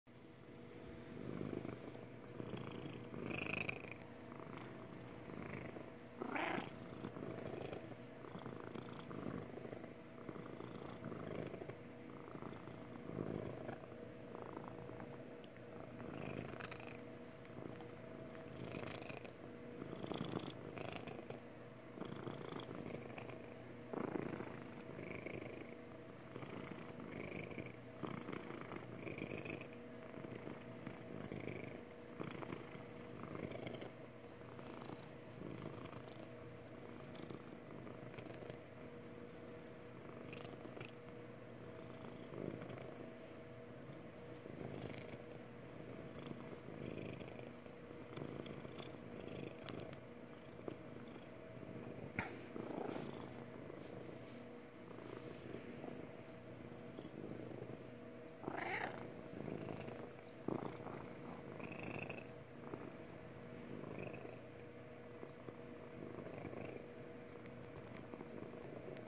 Magic the motor boat
Magic purring her happy purr